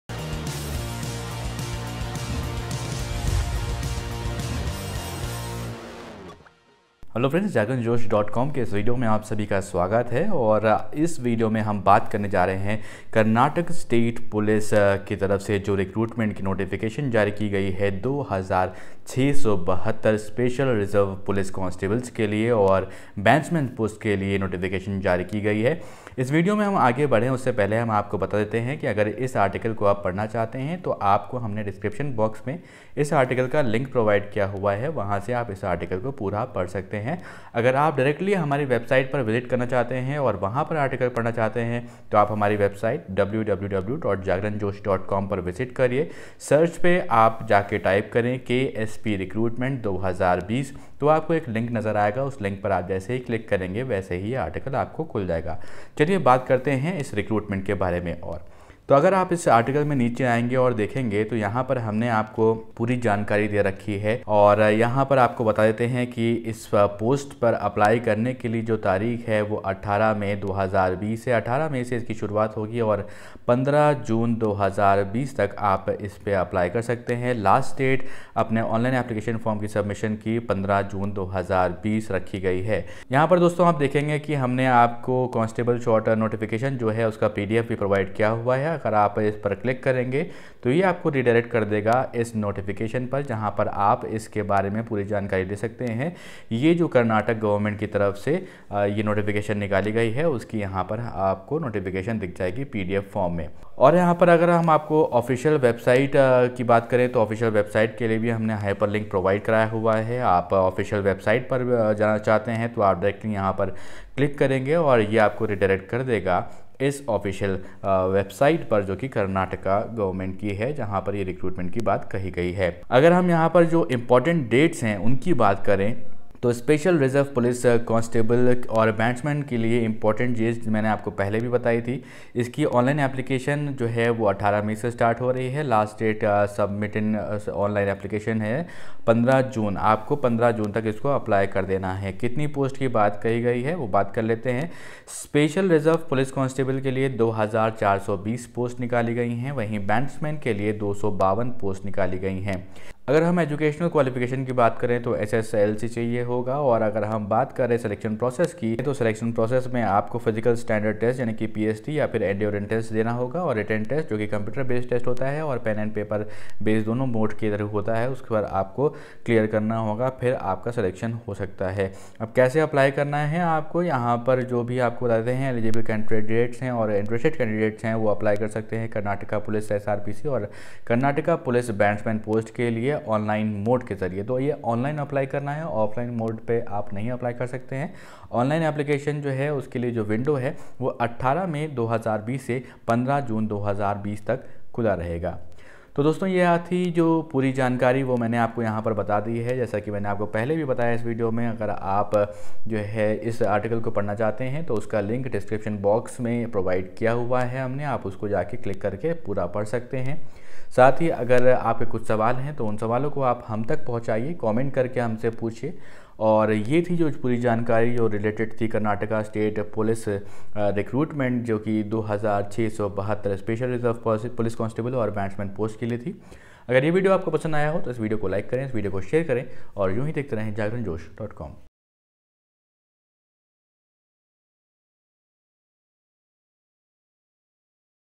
Police Recruitment 2020: 2672 Vacancies announced in KSP, 10th pass can apply
Check out this video to know about Latest KSP Recruitment 2020. Check KSP Recruitment 2020 Vacancies, KSP Recruitment 2020 Eligibility Criteria, Selection Criteria and other details here.